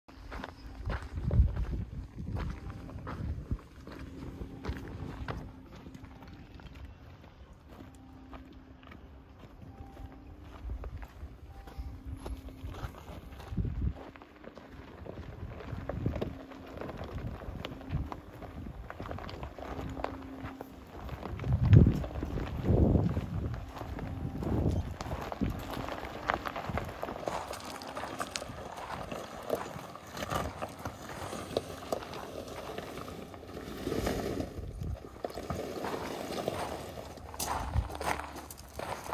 2022 GMC HUMMER EV Super Truck: Quick Off-Roading
The HUMMER EV SUT is an outstanding rock crawler and can hang with the best, whether it's a Jeep Wrangler or a Land Rover. GMC partnered with Goodyear for the truck's standard 35" Wrangler Territory MT (Maximum Traction) tire with an open tread design to help clear out mud. An optional 37” is available from either BF Goodrich or Goodyear. Owners can view the wheels and terrain from an app and UltraVision’s 18 camera angles, including two underbody views to ensure the HUMMER is taking the proper lines off-road. Underbody armory stems from five skid plates and rock sliders for a complete cover. Additional confidence manifests from Terrain Mode, 16" total ground clearance, a 50-degree approach angle, an electric locking differential and rear virtual locking differential, and Air Down Mode to lower tire pressure for better traction. Then you have Extract Mode, which raises the Air Ride Adaptive Suspension another 5.8” to help the truck walk over obstructions or climb out of dicey situations unimpeded. It can also swim through 32” of water. We ascended rocky hills during our desert excursion, bolted through dusty trails, and crawled back down with ease.

Full review = Full review = AutomotiveRhythms.com